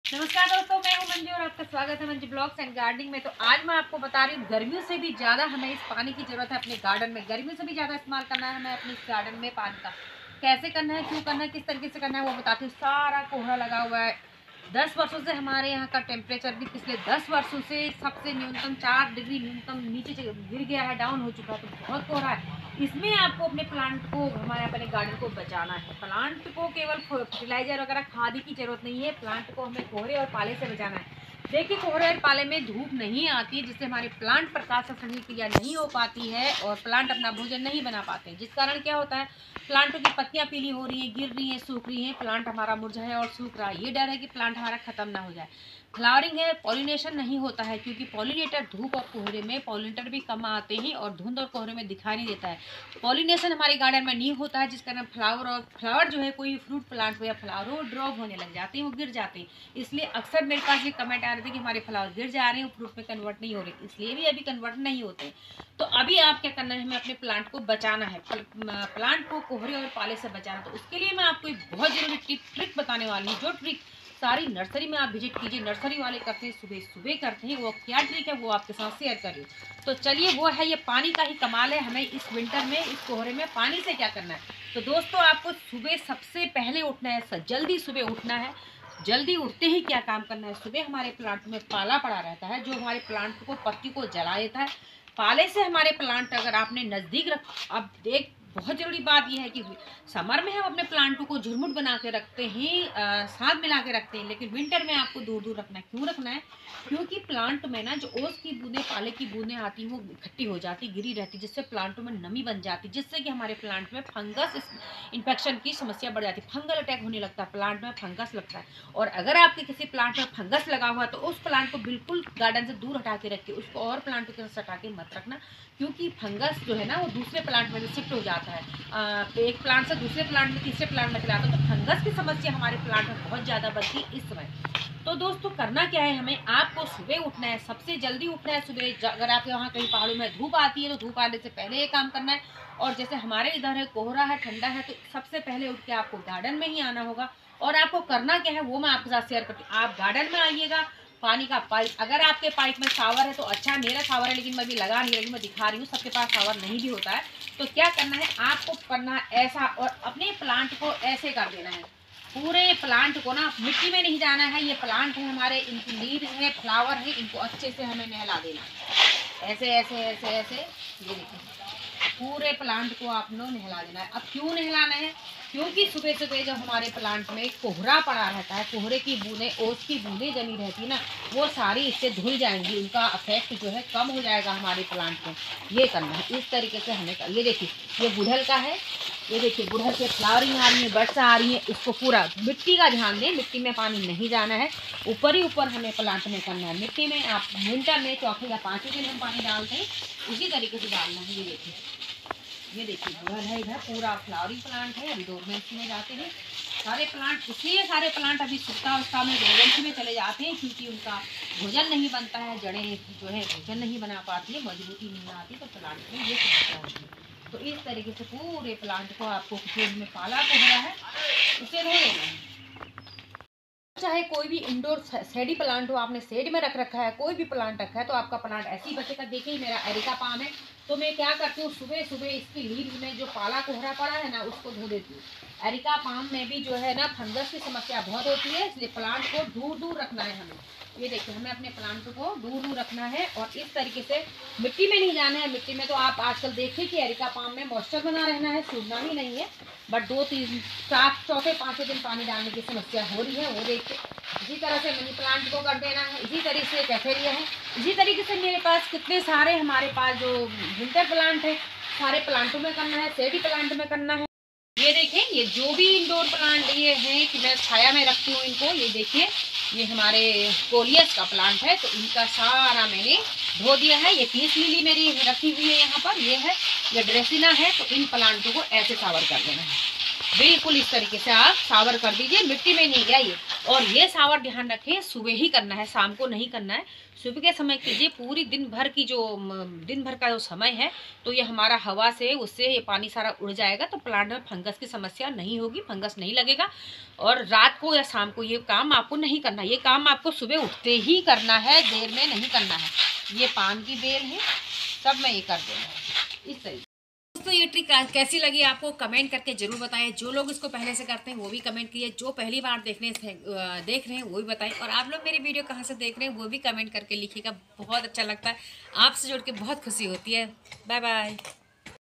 garden winter care // गर्मियों से ज्यादा पानी की जरूरत सर्दियों में गार्डन में होती है जाने कैसे
winter mein garden mein pani dale // pani ka kaise use karna hai jaane /

गार्डन में सर्दियों में पानी की बहुत जरूरत होती है लेकिन किस तरीके से उसे यूज करना है देखें गर्मियों से ज्यादा हम सर्दियों में पानी से अपनी प्लांट को बचा सकते है fertilizer flowers gardening #manju

Manju vlogs and gardening